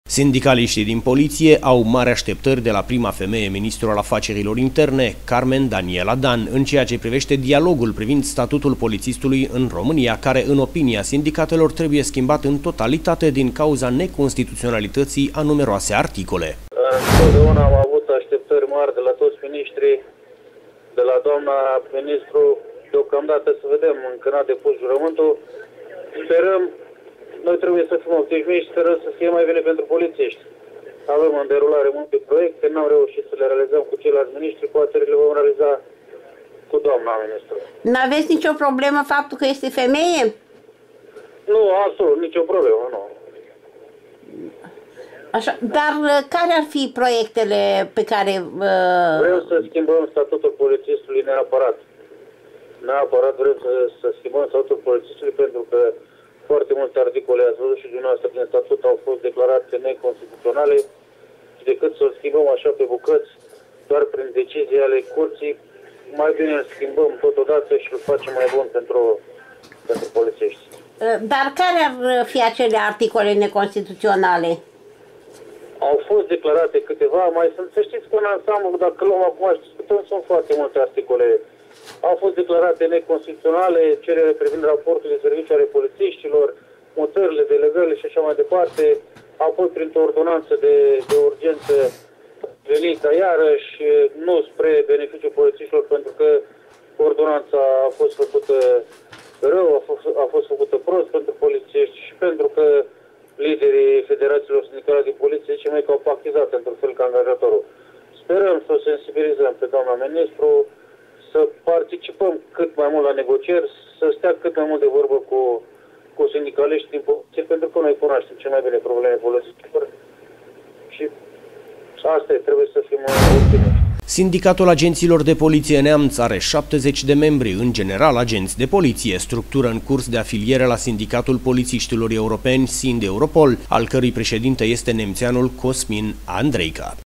MARI AȘTEPTĂRI DE LA PRIMA FEMEIE MINISTRU MAI
Roman24 peste tot! Sindicaliștii din poliție au mari așteptări de la prima femeie ministru al afacerilor interne, Carmen Daniela Dan, în ceea ce privește dialogul privind Statutul polițistului în România, care, în opinia sindicatelor, trebuie schimbat în totalitate, din cauza necostituționalității a numeroase articole.